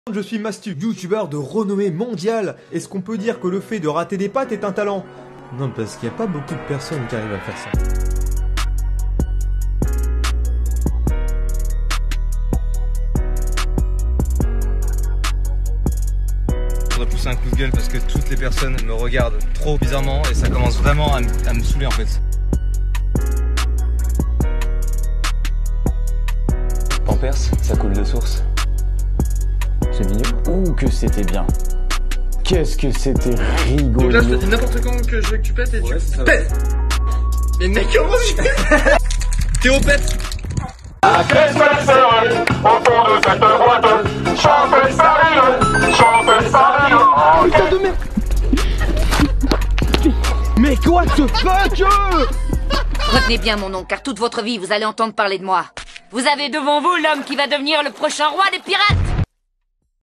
Mastu, le youtubeur humoriste 😉
Mastu s'est fait connaître par un concours organisé par Fanta en collaboration avec Amixem avant de rejoindre la RedBox qui n'aura fait qu'accroître son succès. Drôle et n'ayant pas peur du ridicule, il n'a pas fini de nous amuser ! 😂

Ne rate aucune de nos vidéos en t'abonnant 😉